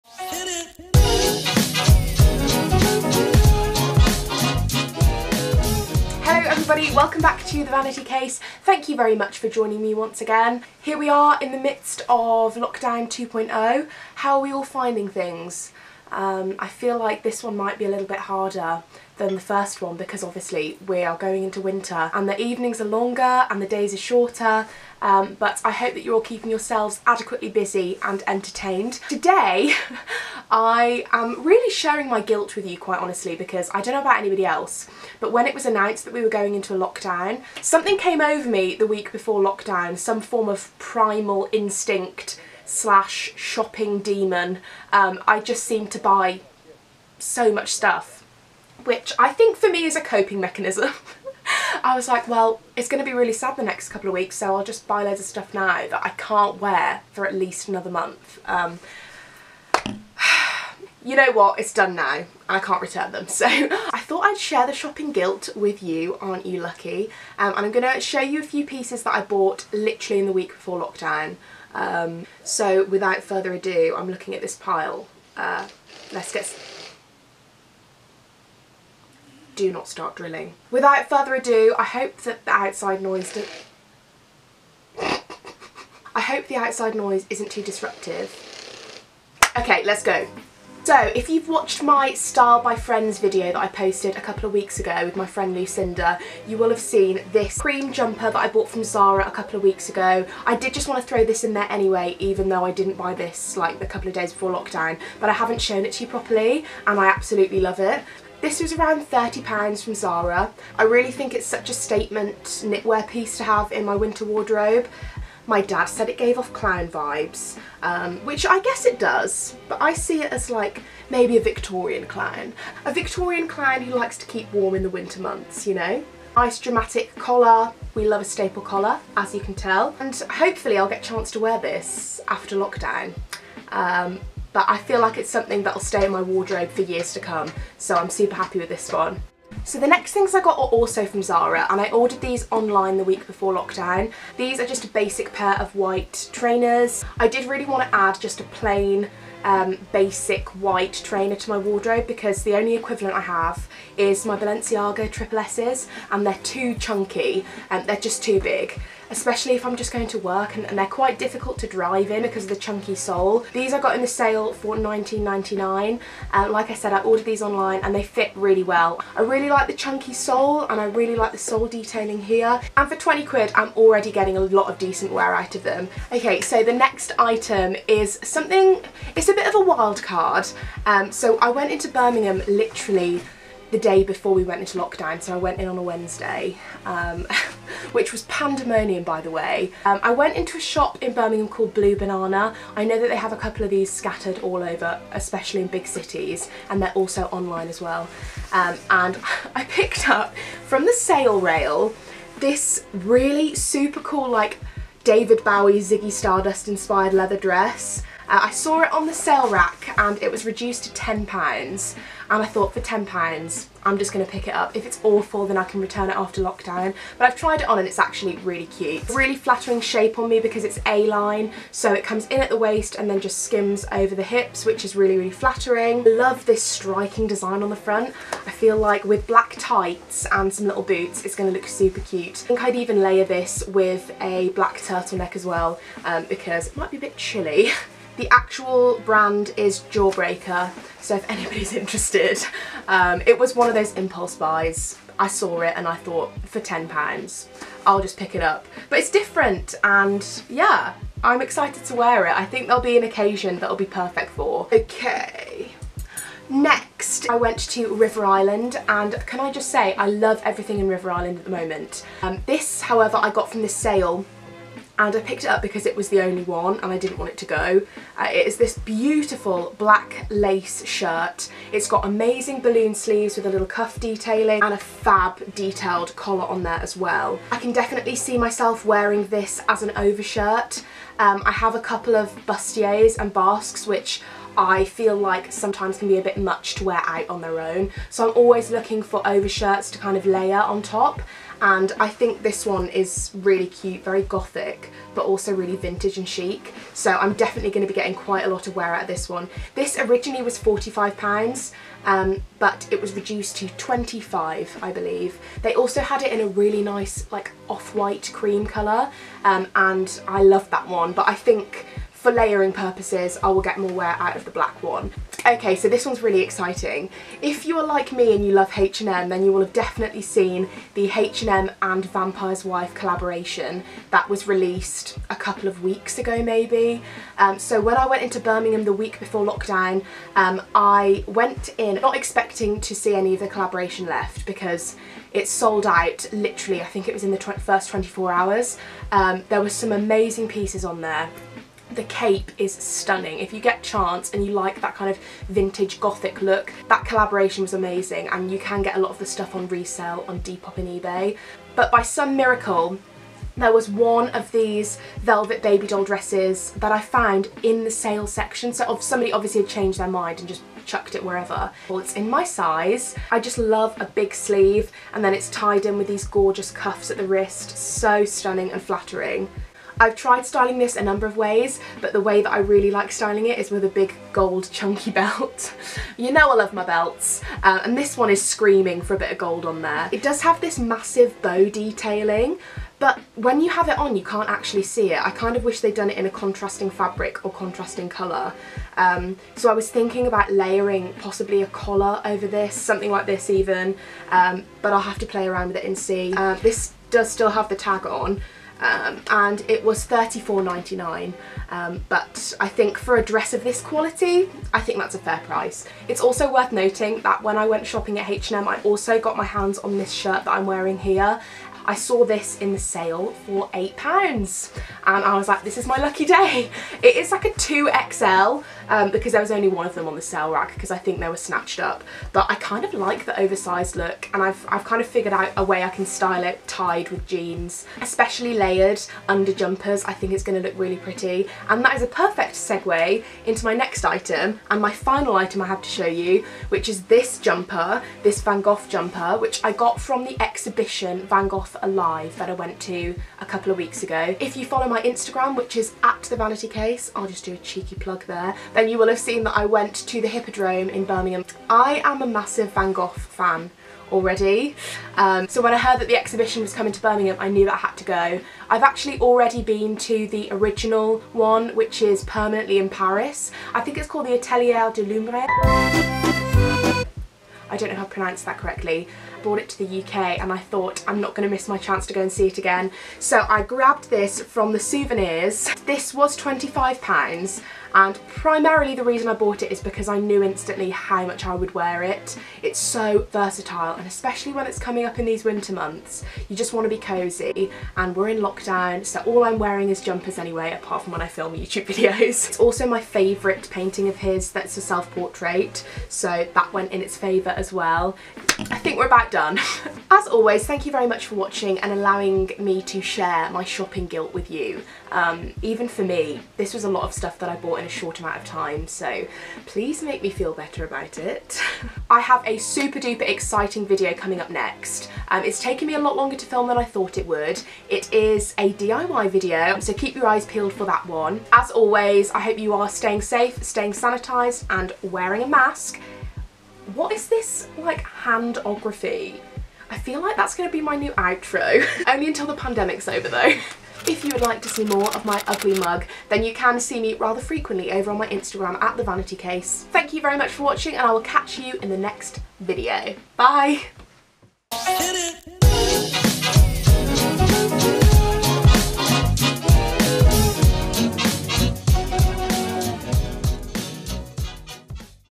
Pre-Lockdown Shopping Splurge!! ¦ THE VANITY CASE
Hello you lovely lot, yes surprise surprise i've spend more money on clothes SHOCK HORROR, right?

Hoping you're all safe & well -- see you in the next!
XOX

00:00 - Intro
01:30 - Zara Frill Jumper
02:19 - Zara Trainers
02:56 - Bowie Leather Dress
04:14 - RI Lace Shirt
05:15 - Vampires Wife X HM Dress
07:11 - Van Gogh Jumper
08:18 - Outro